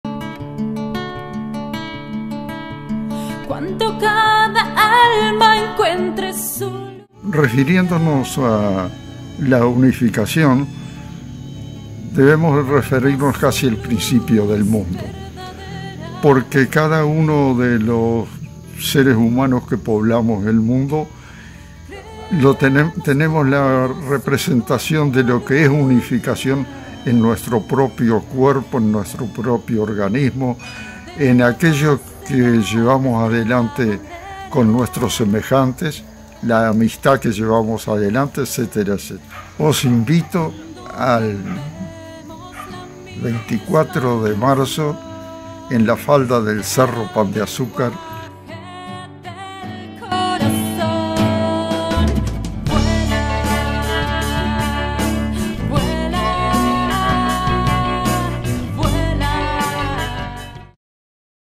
Encuentro de Unificación (Trailer 5)
Desde siempre se ha sabido que la Unidad existe por todo el universo, y que subyace en la diversidad de formas. Los grandes Maestros y pensadores han dado testimonio en más de una oportunidad. Y los descubrimientos científicos están aportando esa prueba, que en cada experiencia y practica nos lleva a entender y comprender el sentido de la Unidad y Universalidad de la naturaleza y el universo.
“Mi Patria como hombre es el Mundo” decía Marco Aurelio.

TE ESPERAMOS EL DOMINGO 24 DE MARZO EN EL CERRO PAN DE AZÚCAR AL 19ª ENCUENTRO DE UNIFICACIÓN !!!